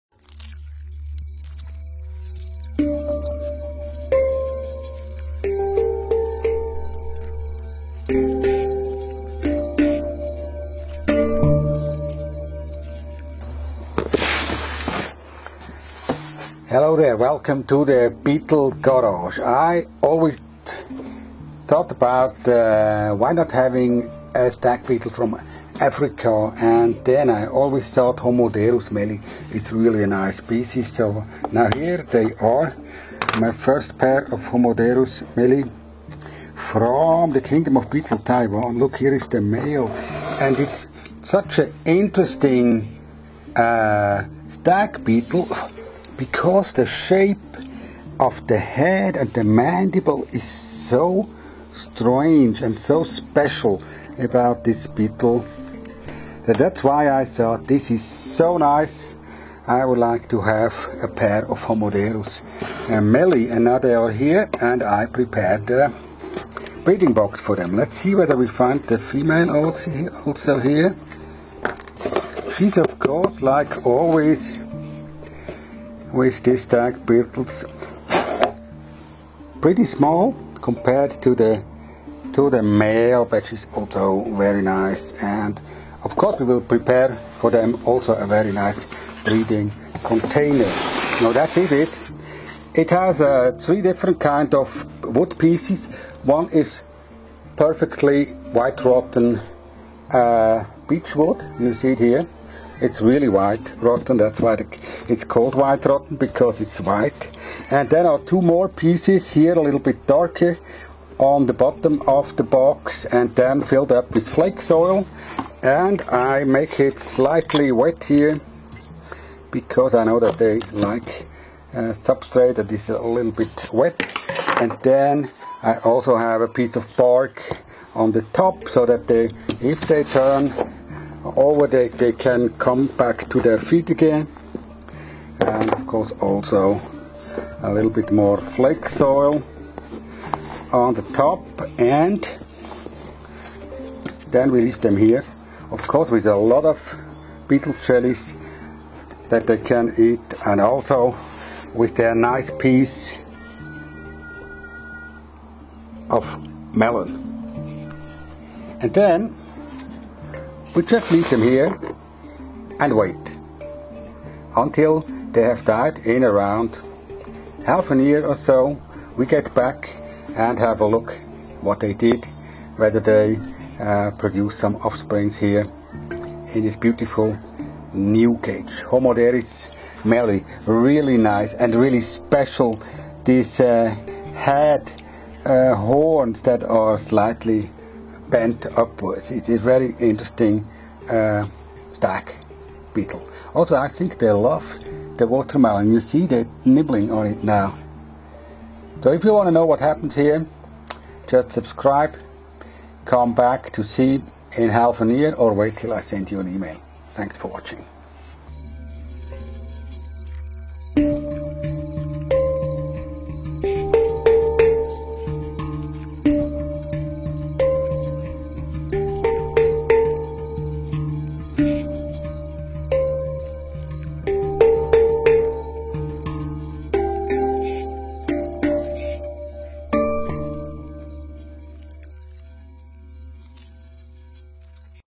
Homoderus mellyi - A pair moves to the breeding box
This african stag beetle was a long time on my christmas list. Let's see whether we manage to present to nthis pair a luxury breeding seeting for their family.